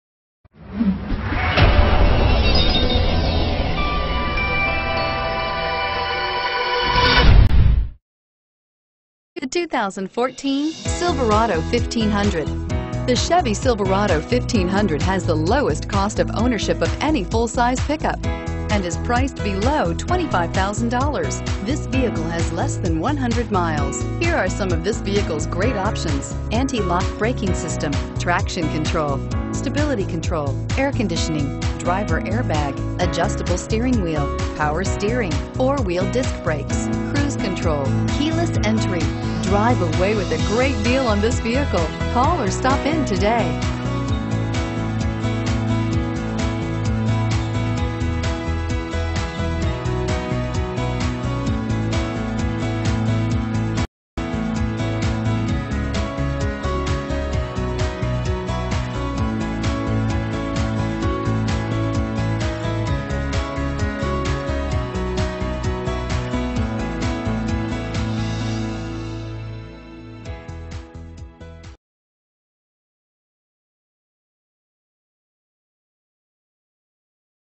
2014 Chevrolet Silverado 1500 Norman OK EZ369124
2014 Chevrolet Silverado 1500

Don't miss this 2014 Chevrolet Silverado 1500. It's equipped with Automatic transmission and features a  exterior. With 3 miles, You'll want to take this car home. Make a great choice today.